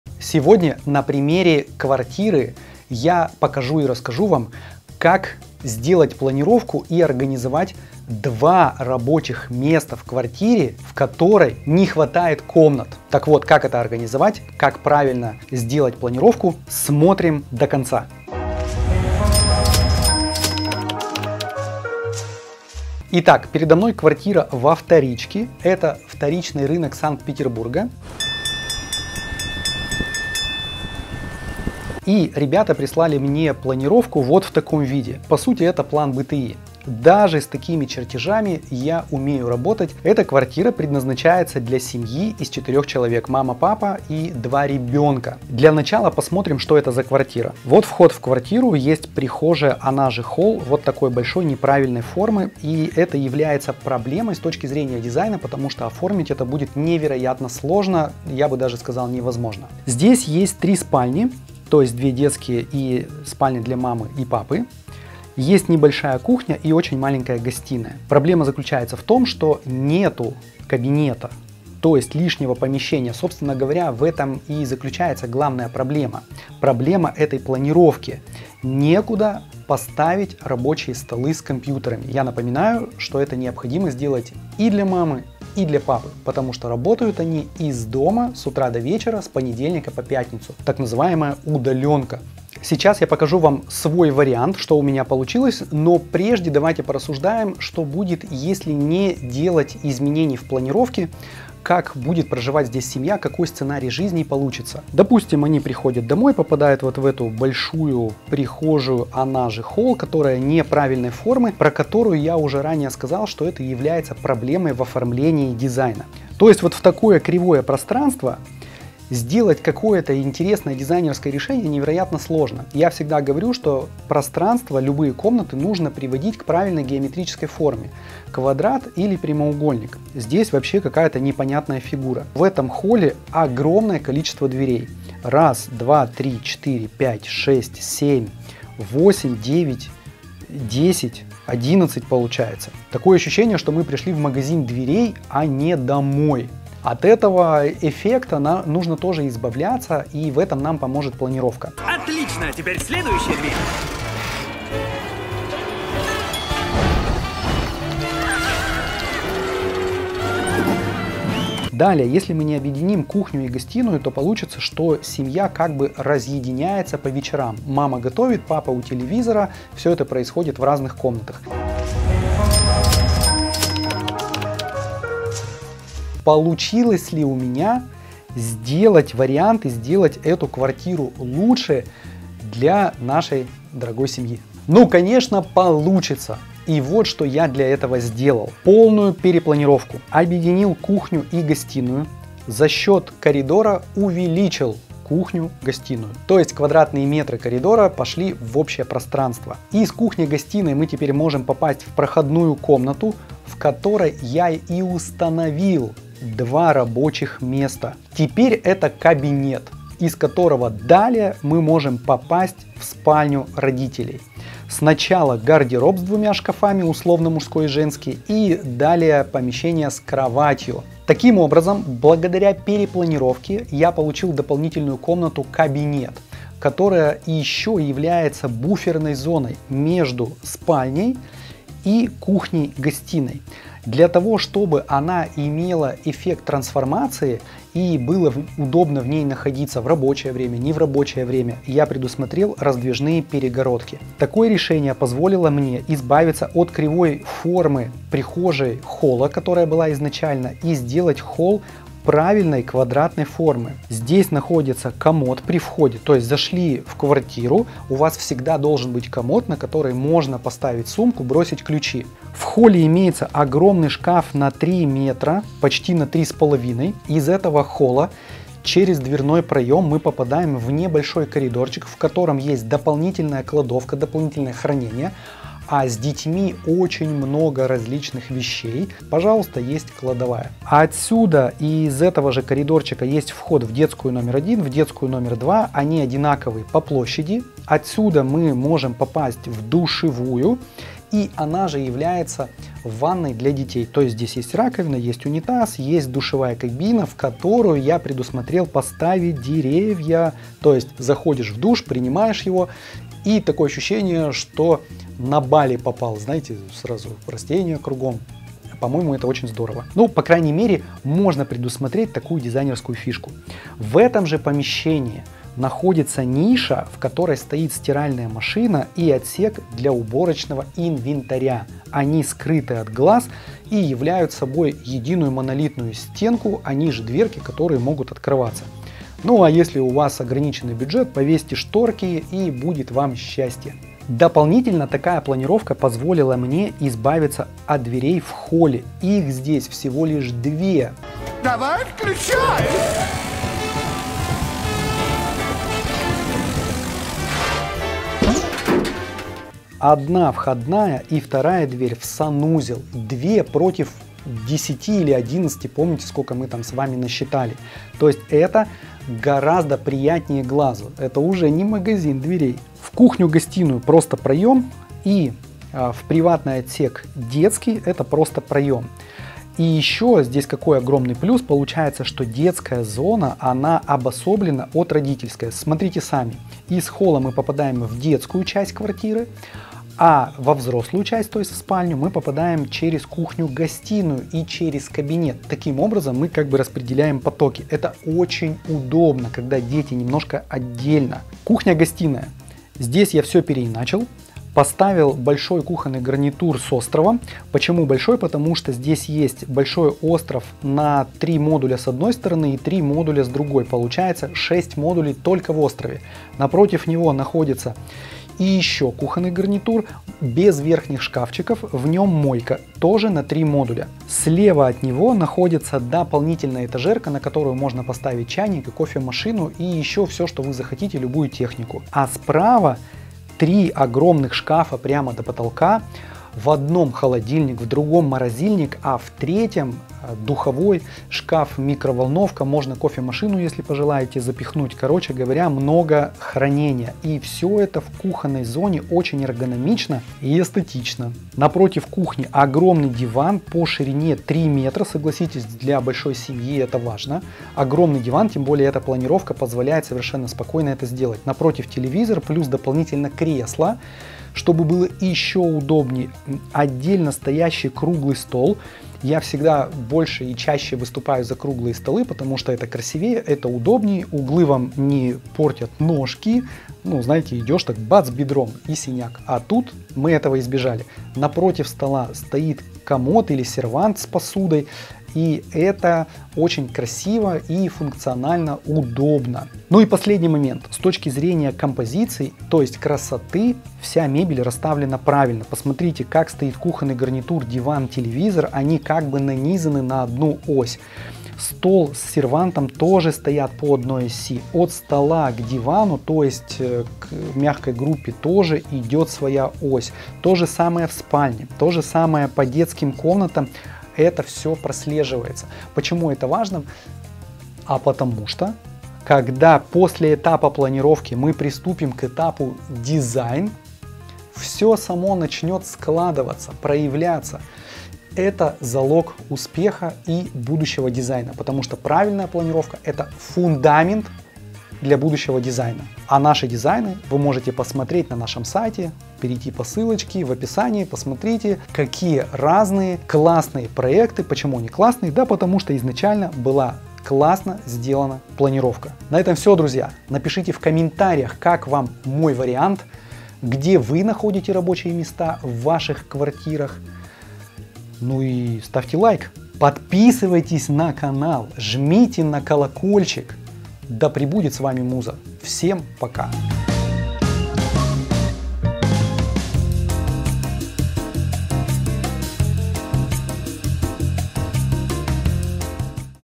Как сделать 2 РАБОЧИХ МЕСТА ДЛЯ РОДИТЕЛЕЙ, когда комнат в квартире НЕ ХВАТАЕТ?
Сегодня на примере квартиры я покажу и расскажу вам, как сделать планировку и организовать два рабочих места в жилье, в котором нет места для рабочей зоны - кабинета. Вы увидите план БТИ квартиры на вторичном рынке Санкт-Петербурга в том виде, какой он есть и я поделюсь с вами, каким образом я сделал правильную композицию. Если у вас 11 дверей в холле и вы не знаете, что с ними делать, видео обязательно к просмотру!

Подробнее смотрите в видео:

00:00 Анонс
00:21 Исходная планировка
02:55 Мой вариант
03:20 2 рабочих места
04:22 Реорганизация холла
06:31 Кухня-гостиная
08:00 Правильная композиция